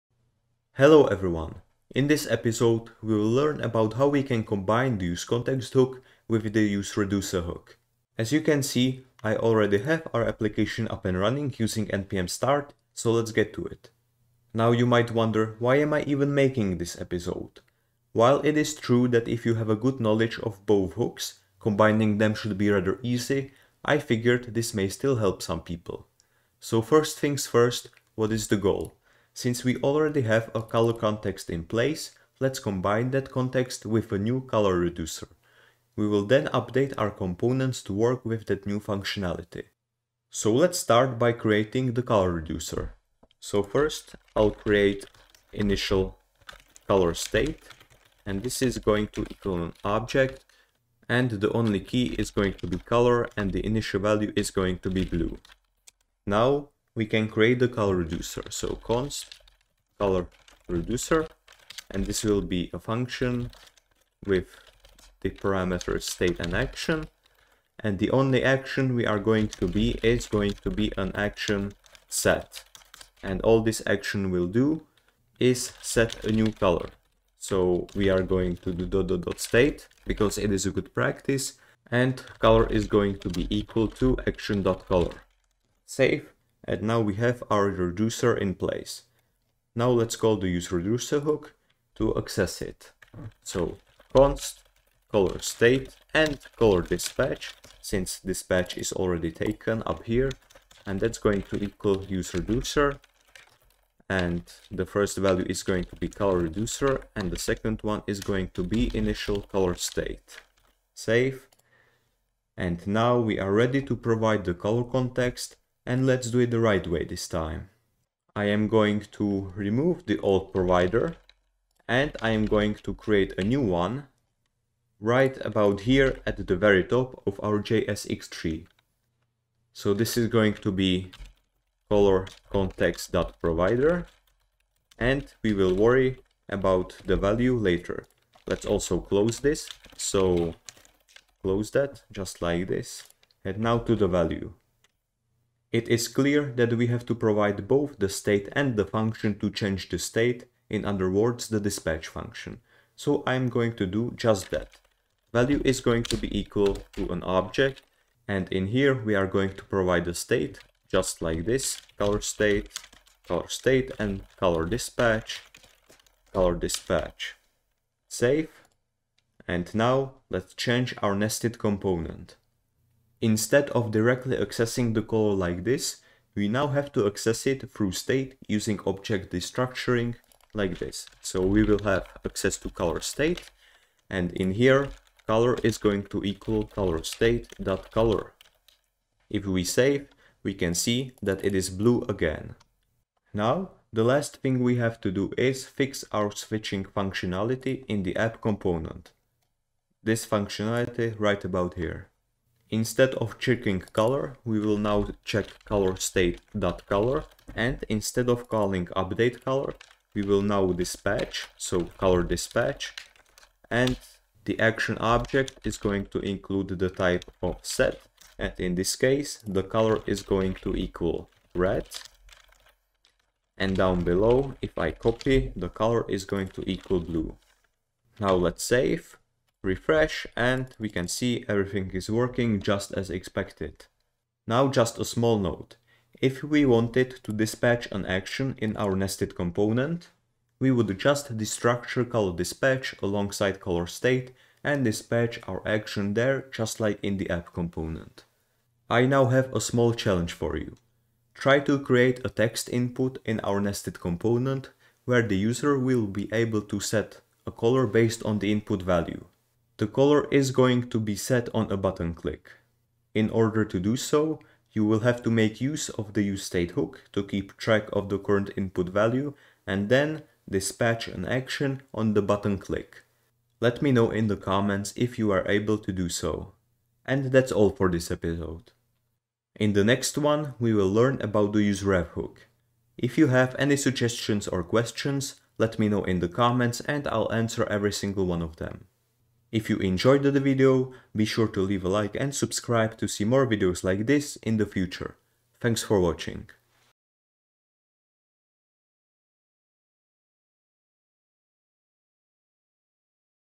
combining useContext with useReducer | React Hooks Tutorial #7
In this episode of my React Hooks Tutorial, we will learn about combining the useContext and useReducer hooks. This may seem quite simple if you have a good knowledge of both hooks, but I believe it may still help some people, because this is one of the best ways of managing state in complex applications, I personally use this method in almost all of my projects.

Timestamps:

►0:00 Introduction
►0:26 Combining the useContext and useReducer hooks
►4:08 Small challenge
►4:36 Ending